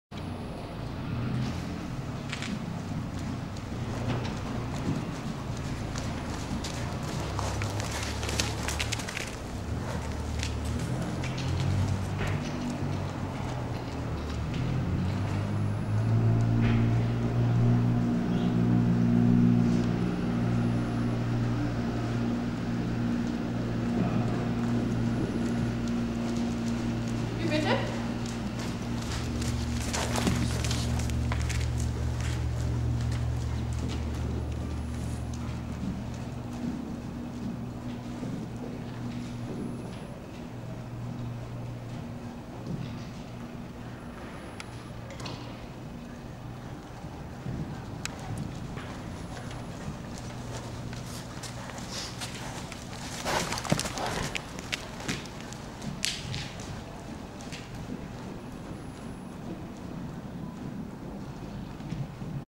Zafriq de Jeu trot
Zafriq de Jeu 6 yr. old 1.85 mt. high S. Rousseau x Jazz x Ulft x Amor, the latter being dam to Sisther de Jeu.